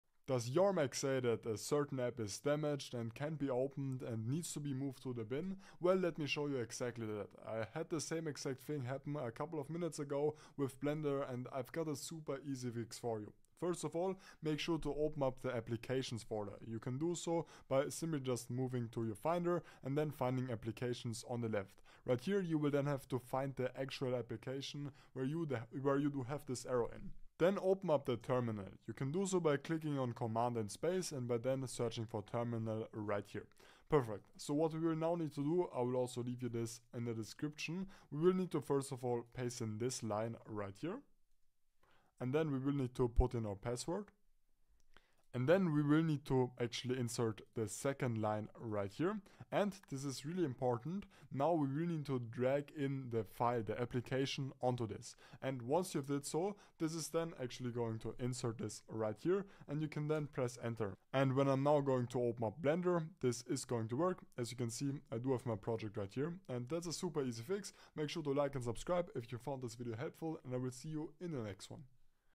How to FIX “is damaged and can’t be opened. You should move it to the Trash” (2026 UPDATED TUTORIAL)
If you're getting the "App is Damaged and cant be opened - move to bin" error on your Mac, don't worry! In this updated tutorial, we'll show you how to fix this issue on macOS. Follow these steps to get your apps up and running smoothly again!

sudo spctl --master-disable
xattr -cr